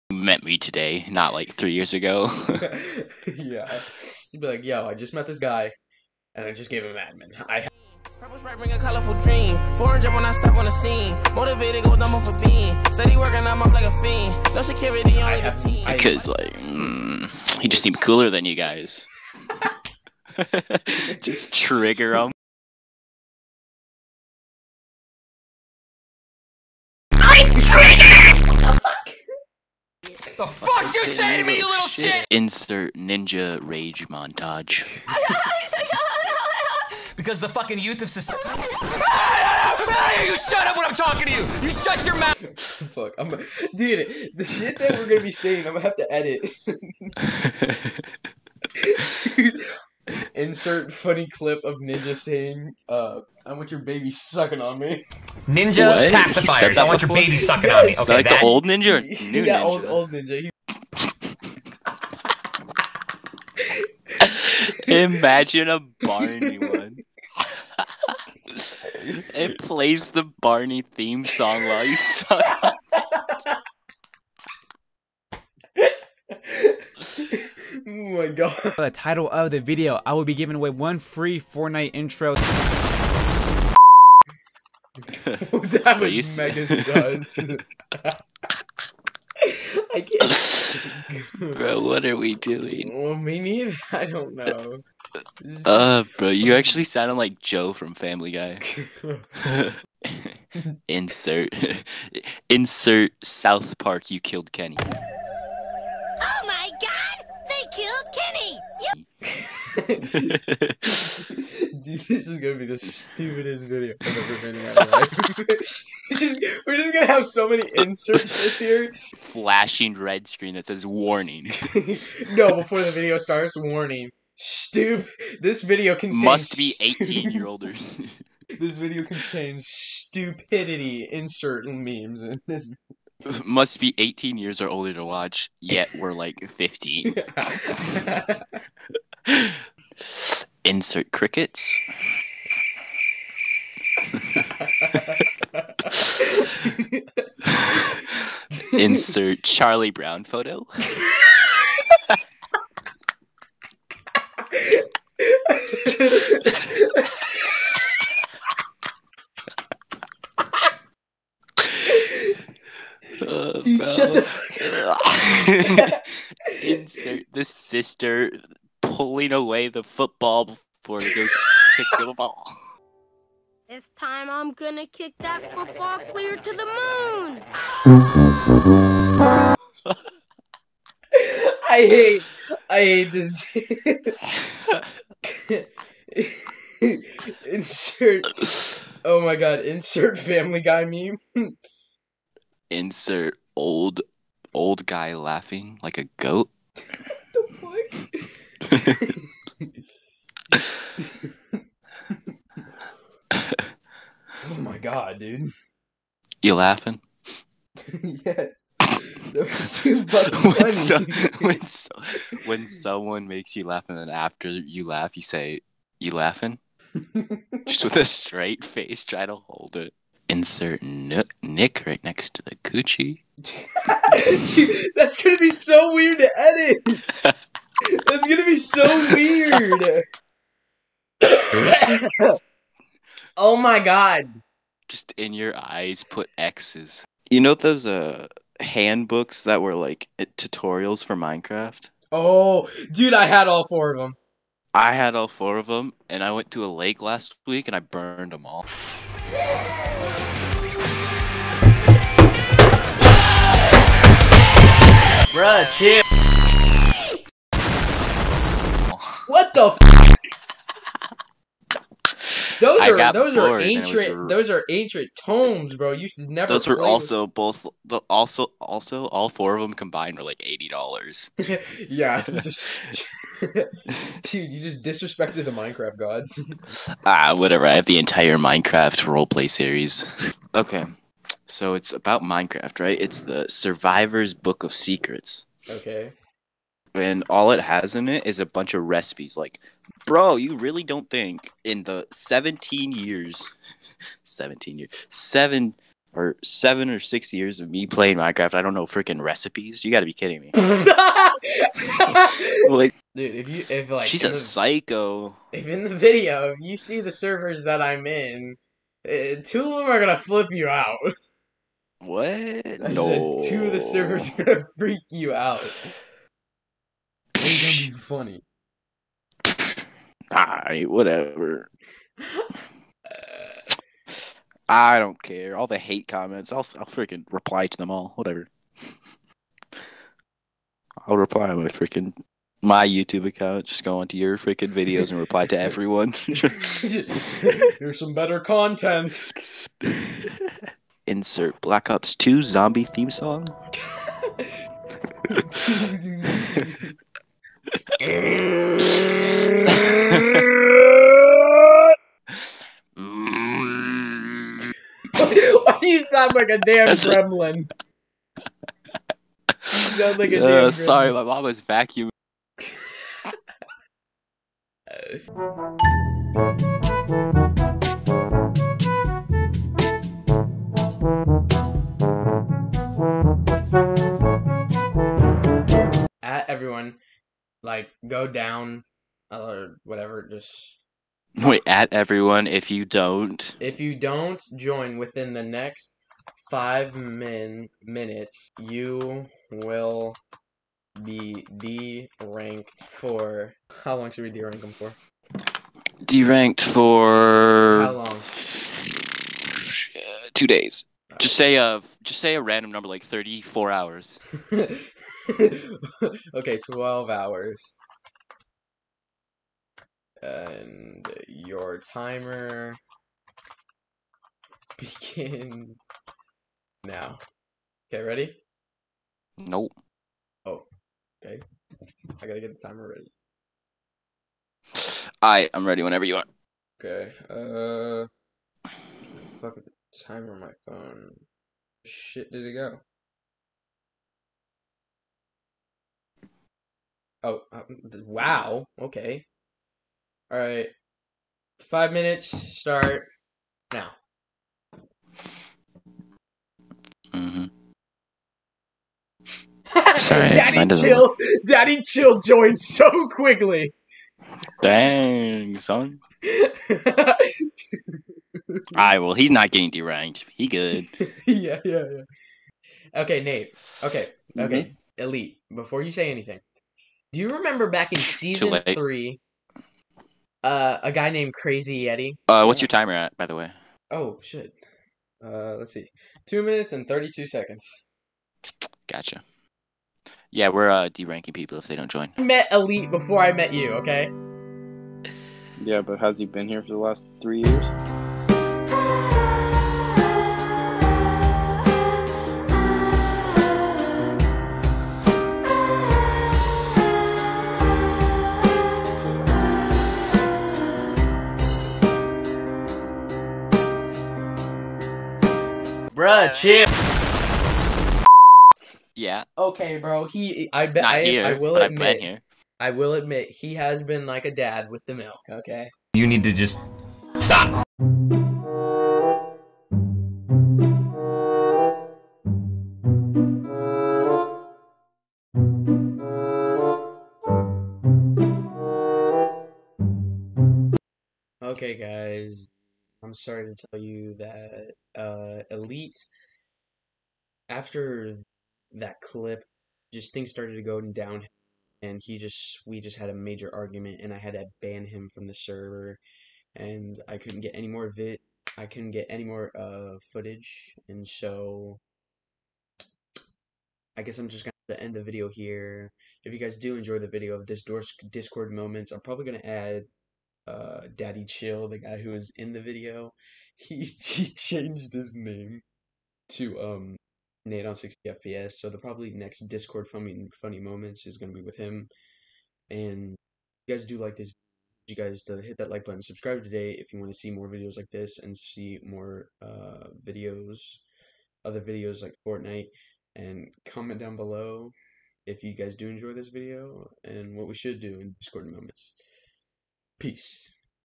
Discord Funny Moments | Things took a Twist
Yo guys in this video me and my friend that i haven't talk to in a while started having fun in Discord and I wanted to show how much fun we had

Sorry no links today fixing things on soicals